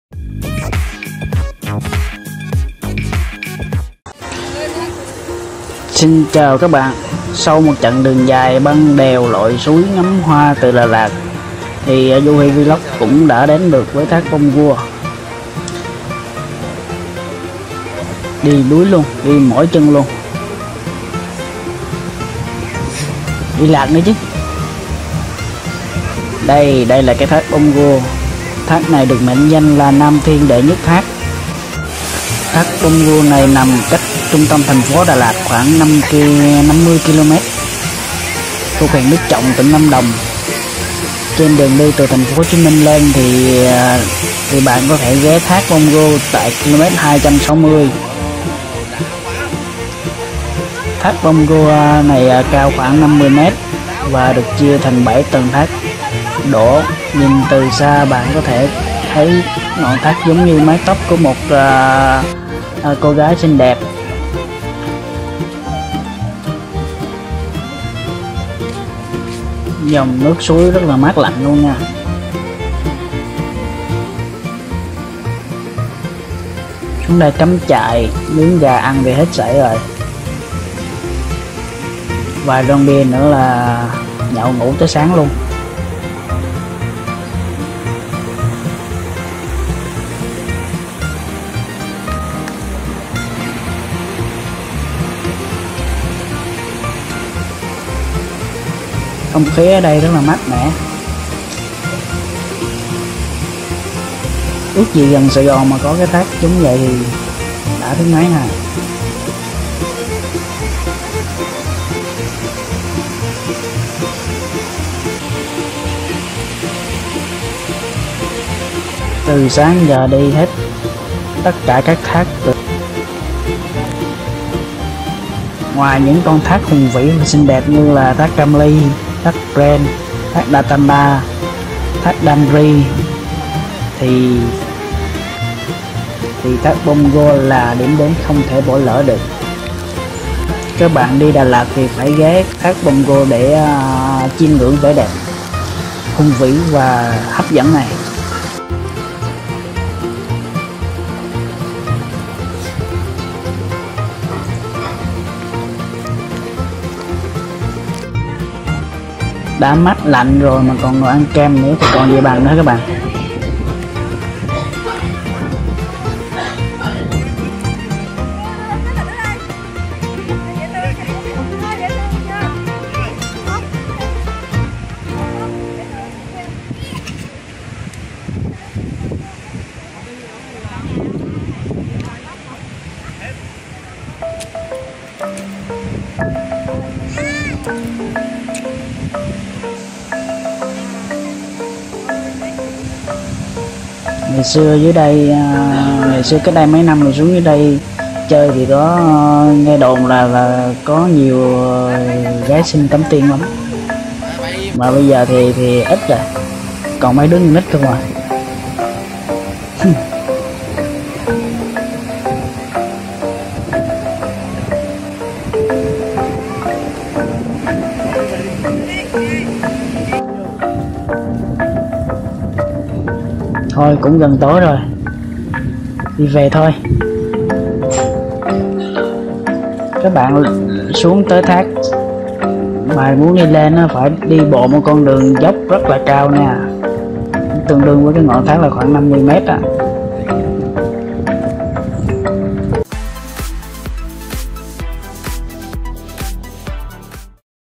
Thác Pongour cảm nhận vẻ đẹp mê hồn và hùng vĩ của nam thiên đệ nhất thác
Du lịch đến Thác Pongour - Vẻ đẹp thiên nhiên hùng vĩ của núi rùng Lâm Đồng
Cảm nhận không khí mát lạnh trong lành của Đà lạt, tránh xa thành phố ồn ào khói bụi, đây là địa điểm không thể bỏ lỡ khi đi du lịch Đà Lạt